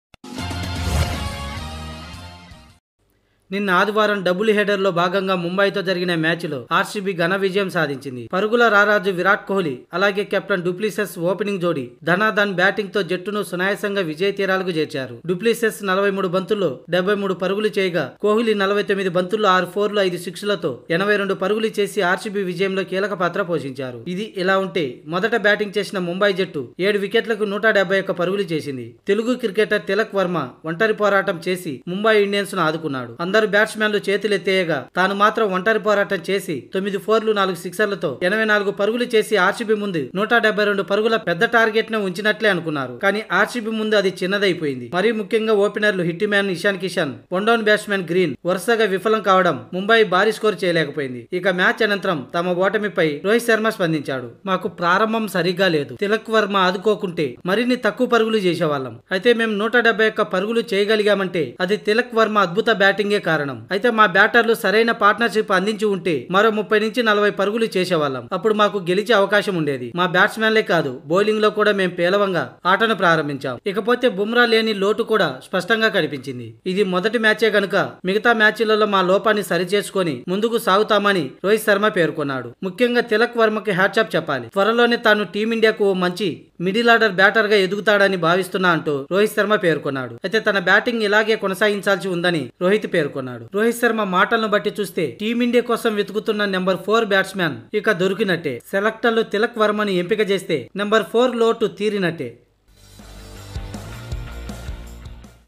Rohit Sharma key comments on Mumbai defeat against RCB | RCB vs MI in IPL 2023
Rohit Sharma key comments on Mumbai defeat against RCB | RCB vs MI in IPL 20023